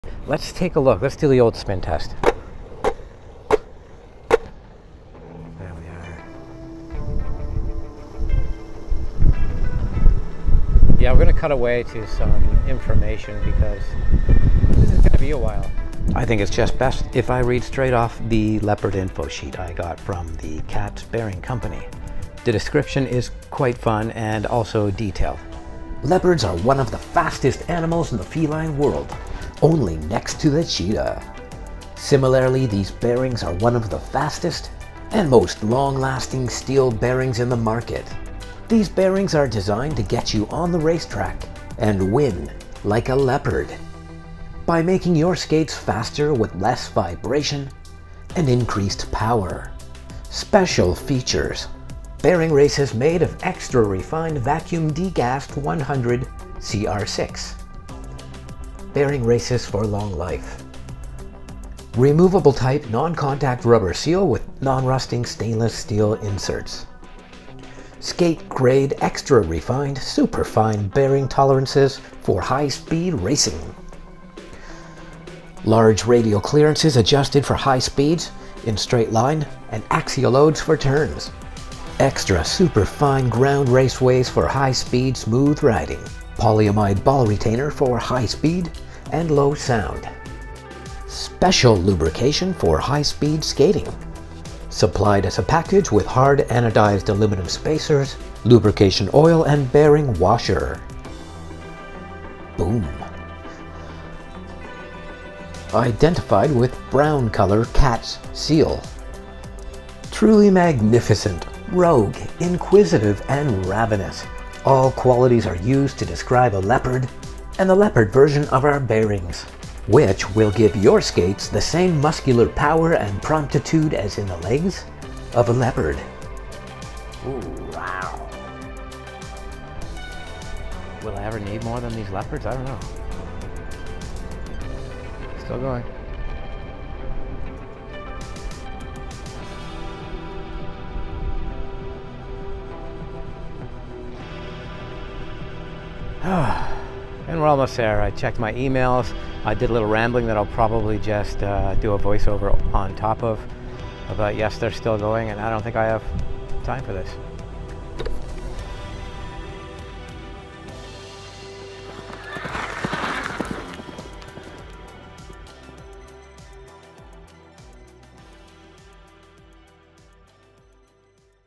Super Crazy Spin -Leopard Bearing Spin Test (from review)
Inline skate bearing free spin test is extraordinary.

City Skater Bill Stoppard shows you the spin and reads the specs of the Leopard Bearings Shot in the  Queen Street West district in Toronto.